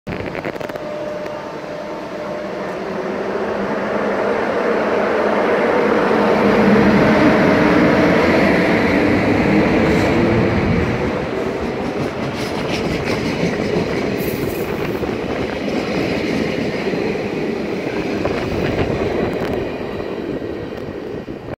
DD-AR Vertrek Bilthoven
Stam 7317 met loc 1733,laatste dag DD-AR in serie 5600